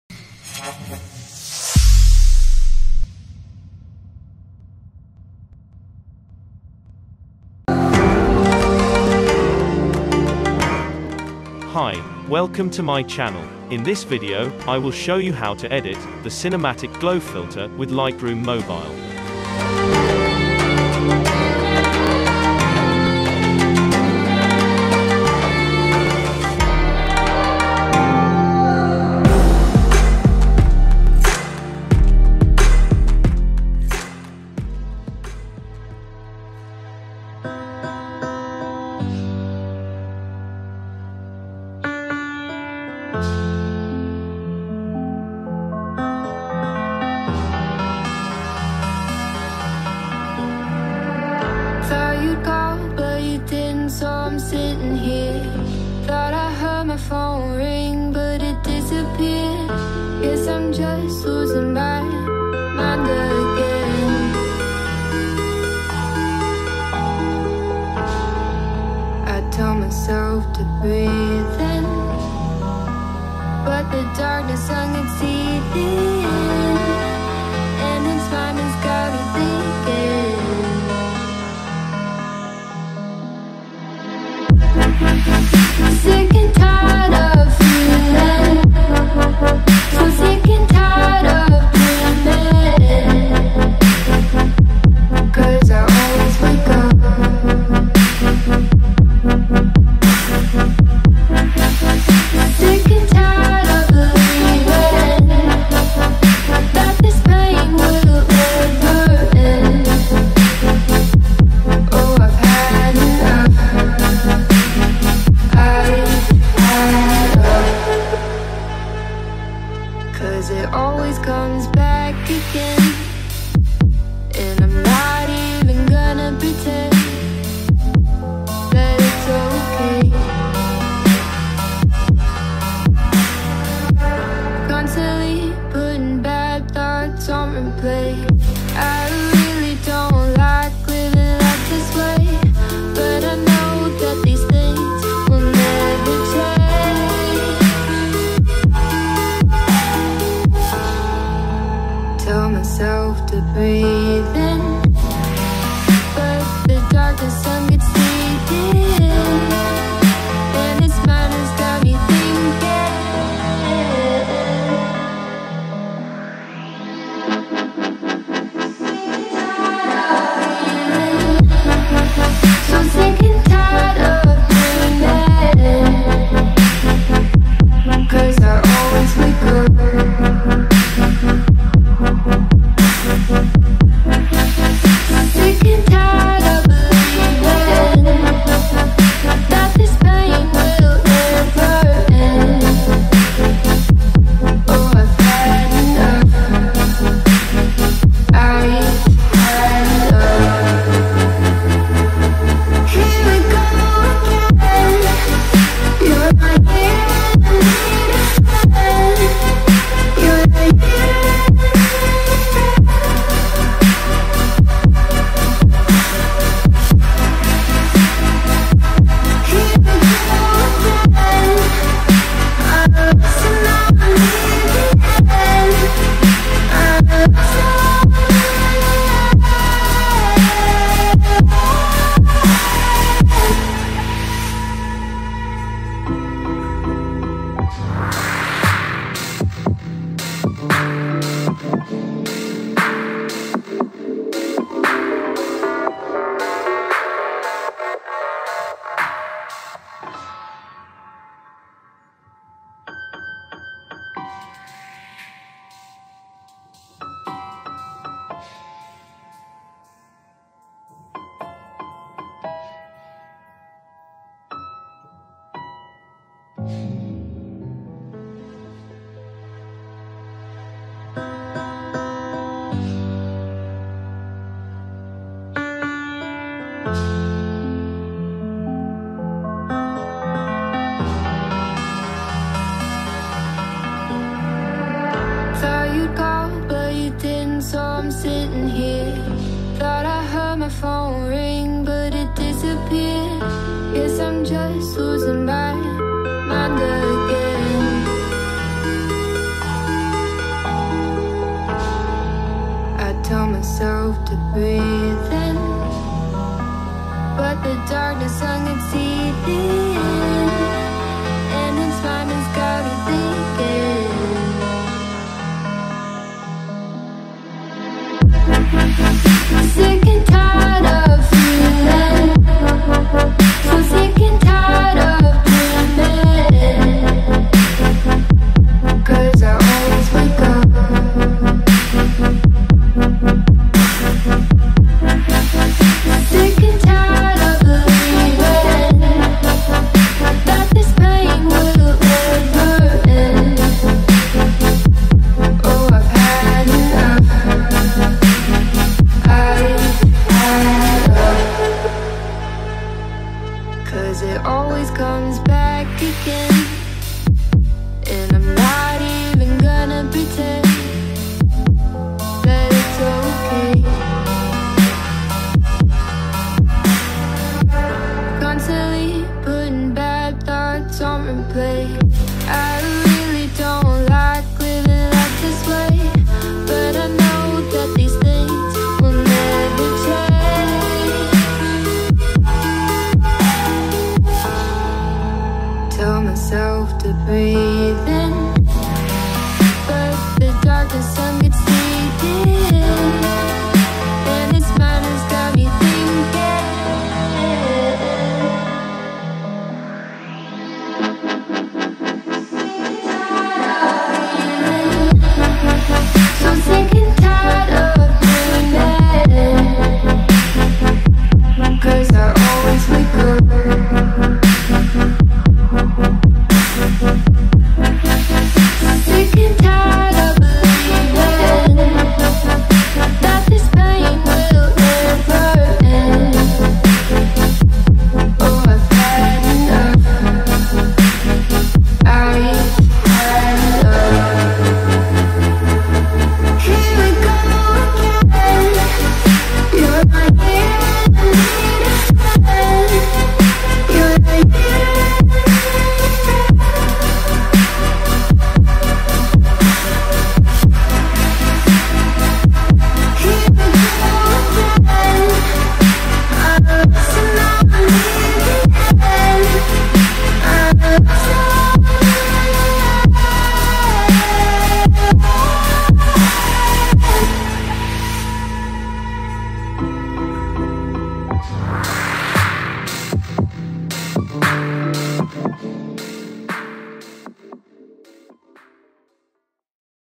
Cinematic Glow presets - Lightroom Mobile presets - Free DNG
Free lightroom presets, lightroom mobile tips.
 How to edit the cinematic glow preset? How are cinematic glow photos arranged? Edit with Lightroom.
The answer to all questions is in this video 😉

Three free presets I've edited in the video
                Cinematic Glow Presets

If you want to buy premium presets in the package
                      AD Preset Store

Tags